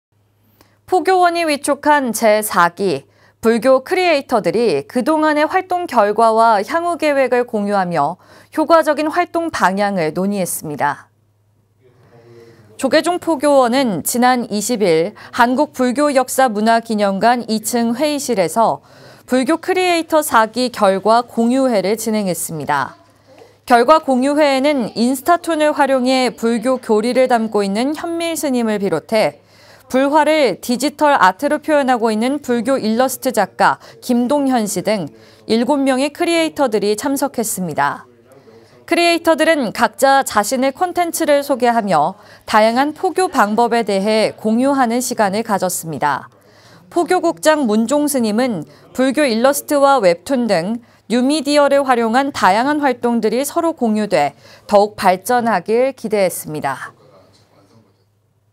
[BTN뉴스] 제4기 불교크리에이터 결과 공유회‥포교방법 다양화
포교원이 위촉한 제4기 불교크리에이터들이 그동안의 활동결과와 향후 계획을 공유하며 효과적인 활동방향을 논의했습니다.

조계종 포교은 지난 20일 한국불교역사문화기념관 2층 회의실에서 ‘불교크리에이터 4기 결과 공유회’를 진행했습니다.

결과 공유회에는 인스타툰을 활용해 불교 교리를 담고 있는 현밀스님을 비롯해 불화를 디지털 아트로 표현하고 있는 불교일러스트 작가 김동현 씨 등 7명의 크리에이터들이 참석했습니다.

크리에이터들은 각자 자신의 콘텐츠를 소개하며 다양한 포교 방법에 대해 공유하는 시간을 가졌습니다.

포교국장 문종스님은 불교일러스트와 웹툰 등 뉴미디어를 활용한 다양한 활동들이 서로 공유돼 더욱 발전하길 기대했습니다.